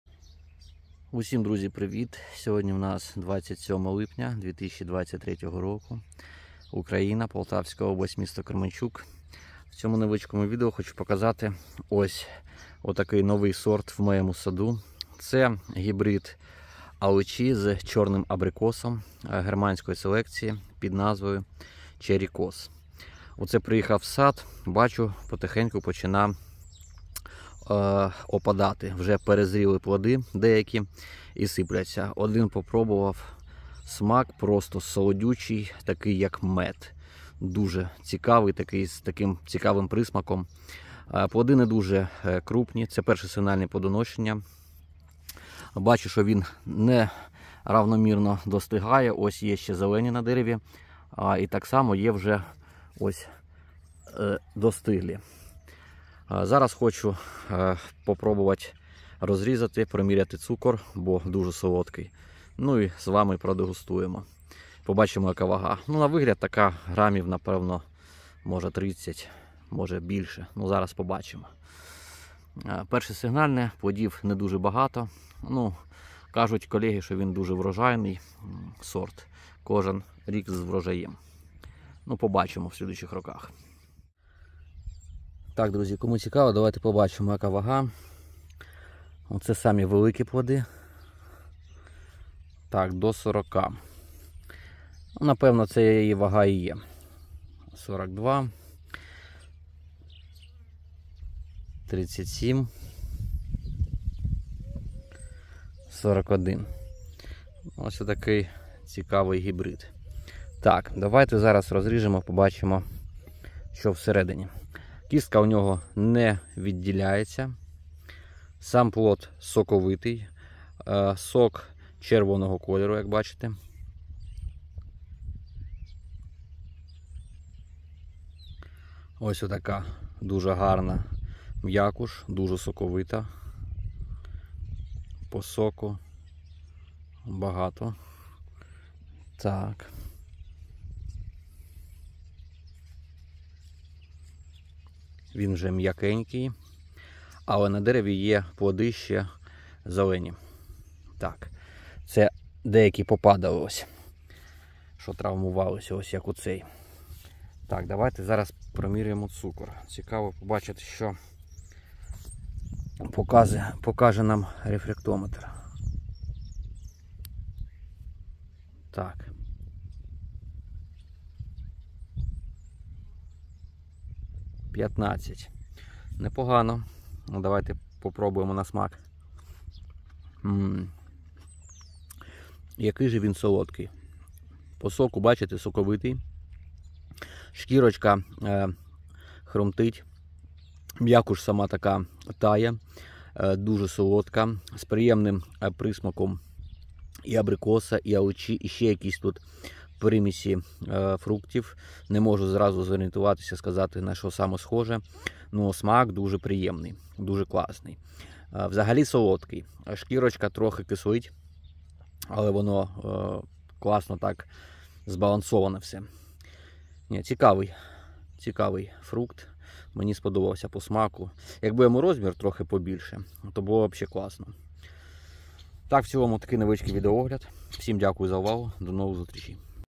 Алича гібрідная Черрікос
Черикос - це гібрид чорного абрикосу та аличі Німецької селекції.
Інформації процей сорт мало ,будемо спостерігати і ділитися інформацією з вами.